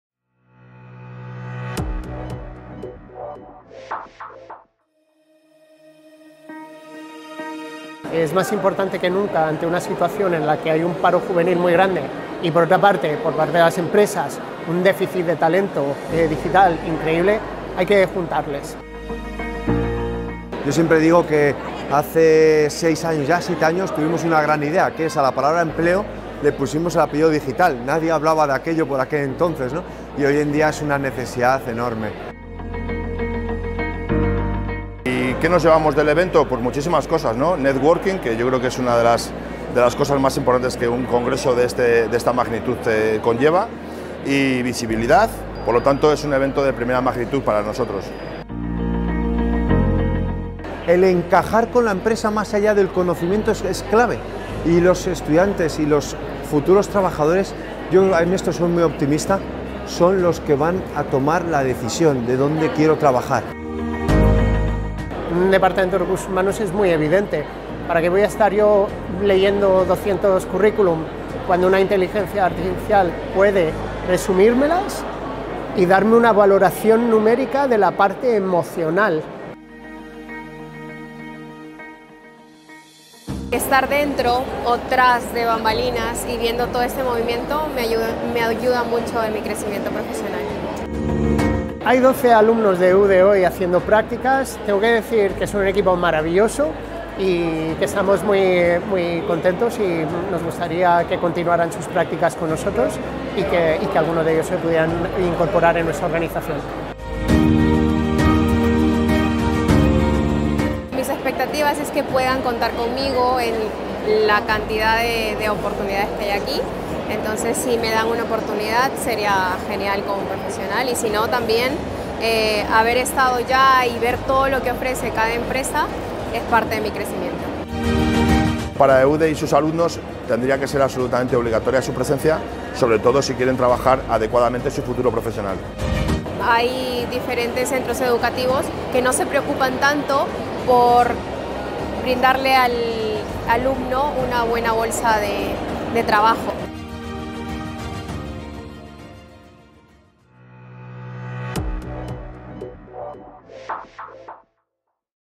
EUDE Business School en FEED2023
El lunes pasado nos acercamos hasta La Nave, en Villaverde Alto, para documentar cómo se vive y potencia este evento de networking. Pudimos hablar con varios conferenciantes, así como con una alumna de EUDE que estaba realizando sus prácticas laborales en este FEED2023.